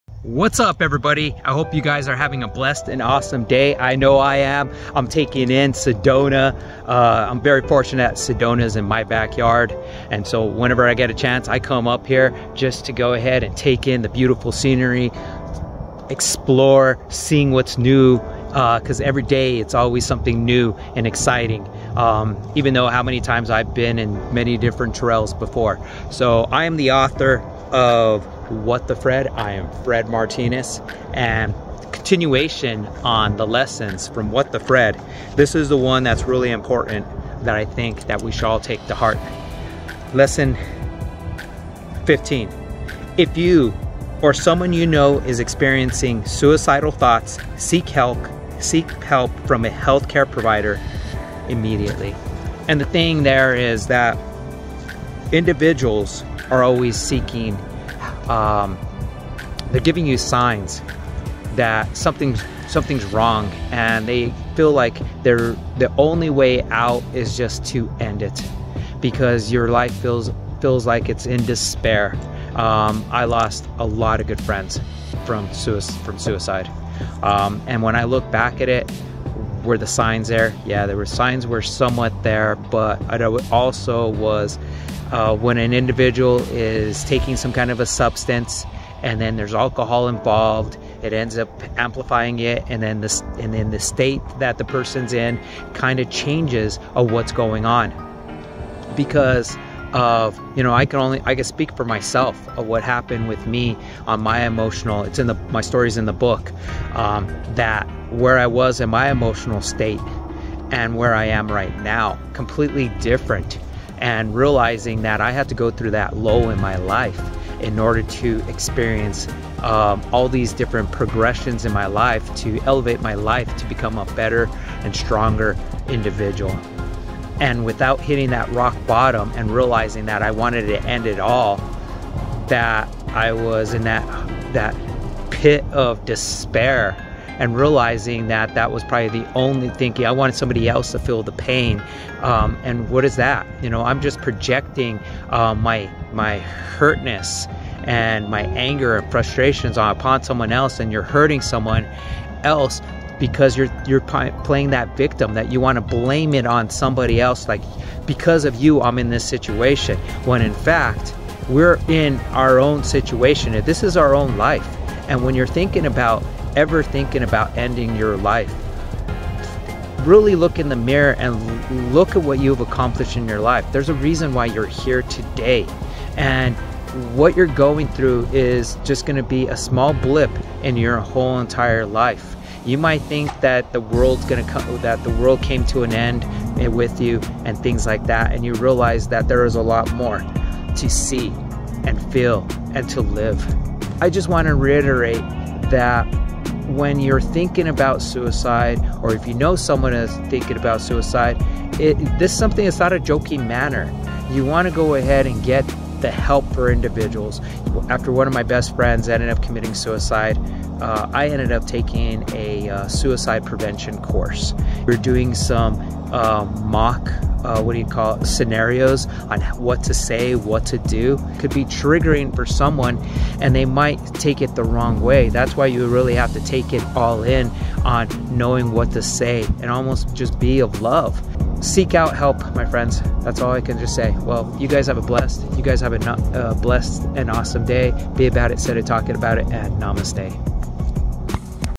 Lesson 15: Support and Hope- Seeking Help for Suicidal Thoughts Part I
Lesson 15 addresses a critical topic: if you or someone you know is experiencing suicidal thoughts, seeking help is crucial. This video emphasizes the importance of reaching out to a healthcare provider immediately. We discuss the significance of mental health awareness, removing the stigma surrounding seeking help, and providing support for those going through difficult times. This video aims to be a resource and a reminder that help is available and there is hope.

Video/Music made using Filmora